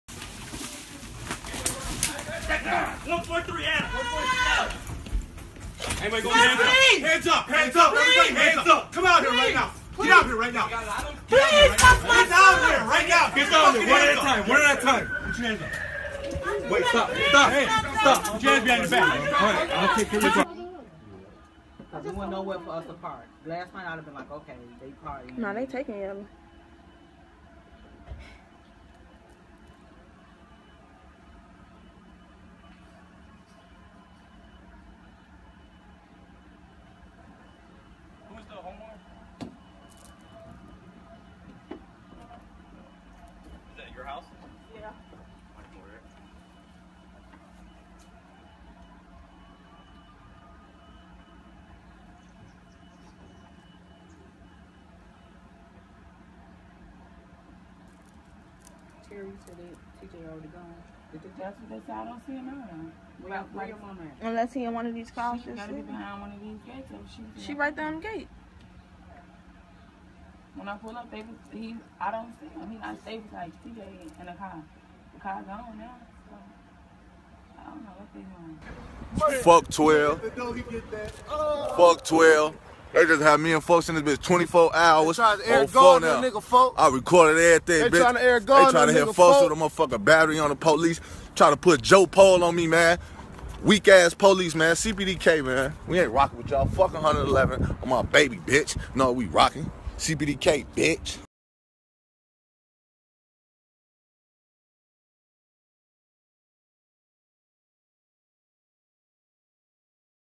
King Yella harassed & arrested by Chicago police ( Gun charges )
" Arrested for license gun "